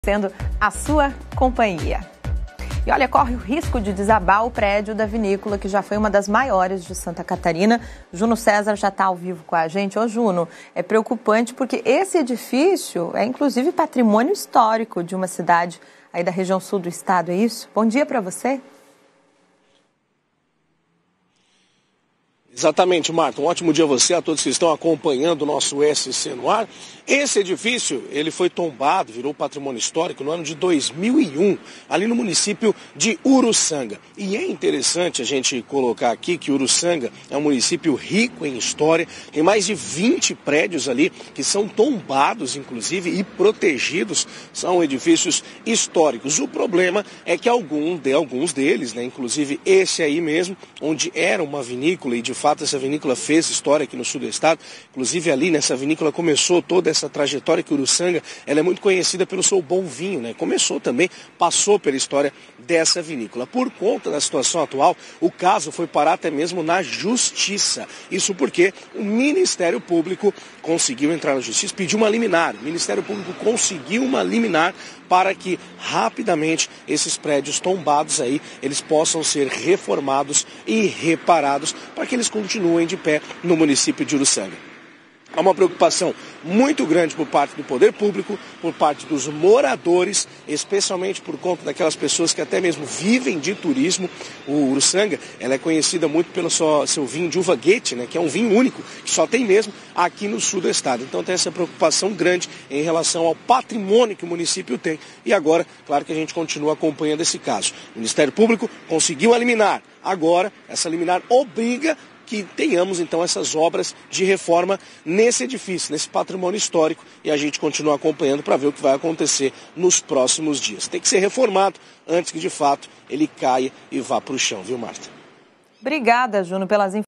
Justiça autoriza reparos em estrutura de prédios tombados na cidade de em Urussanga
Acompanhe os detalhes dessa decisão judicial proferida graças a ação do Ministério Público de Santa Catarina.